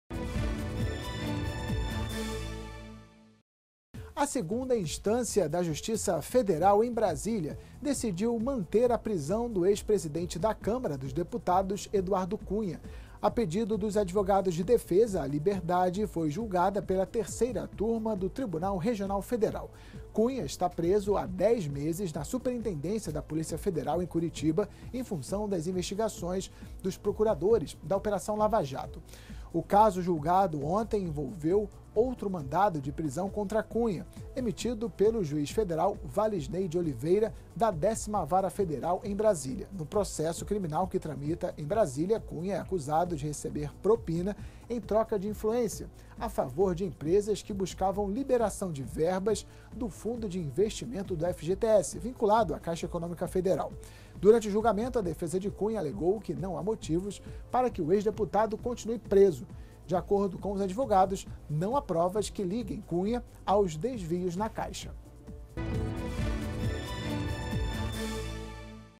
Justiça Federal em Brasília decide manter a prisão de Eduardo Cunha
A pedido dos advogados de defesa, a liberdade foi julgada pela Terceira Turma do Tribunal Regional Federal. Cunha está preso há 10 meses na Superintendência da Polícia Federal em Curitiba em função das investigações dos procuradores da Operação Lava Jato.